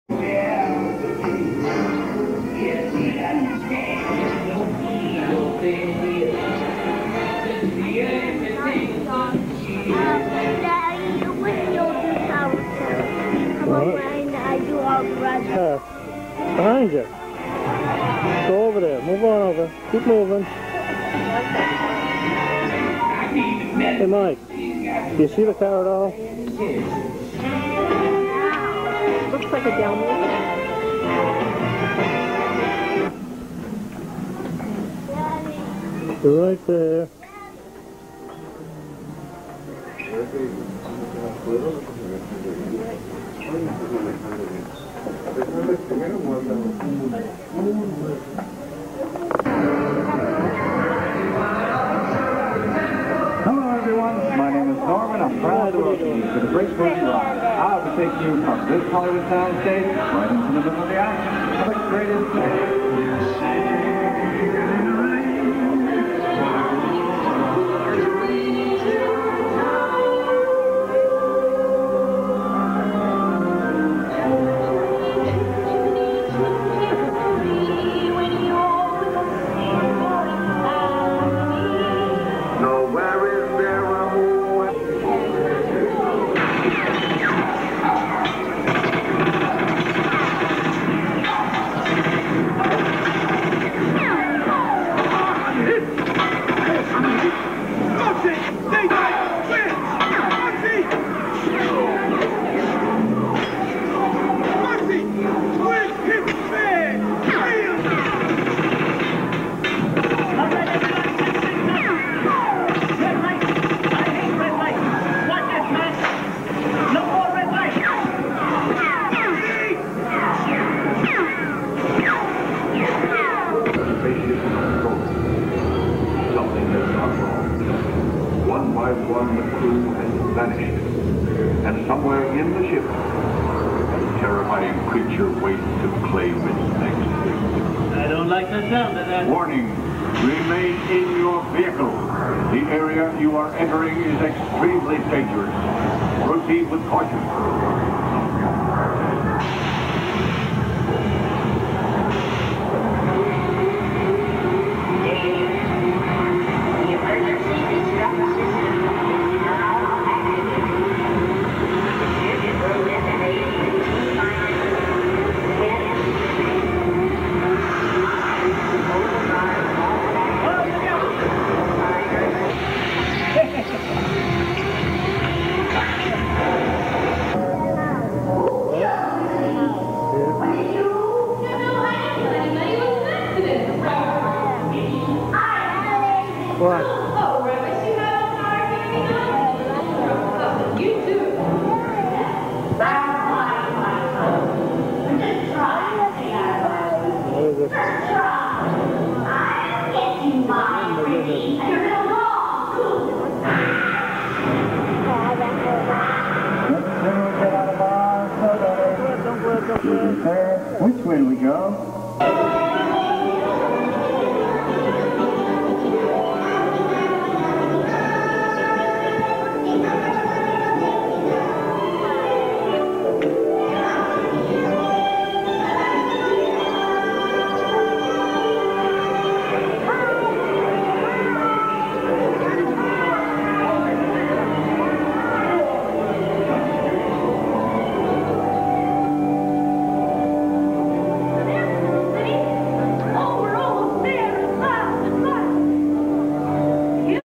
Great Movie Ride Queue + Partial POV (1998), Disney-MGM Studios Dark Ride | Non-Copyright
The Great Movie Ride was one of two opening day rides for Disney-MGM Studios.  This movie-themed attraction took riders past scenes from iconic films- some by Disney and some from other film studios.  This ride was removed for Mickey and Minnie's Runaway Railway, but this video captures some of the queue line and ride experience from back in 1998.